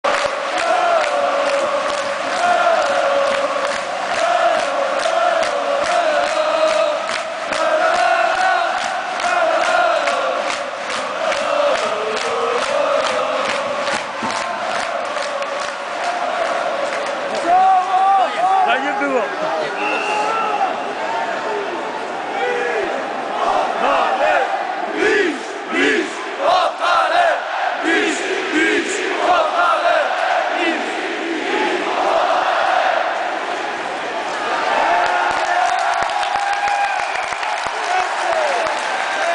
Podhale Nowy Targ - cracovia . Mistrz 2010 Radośc kibiców ,koncówka meczu! 17.03
koncowe fragmenty i i rozpoczete Swietowanie...w koncówce meczu...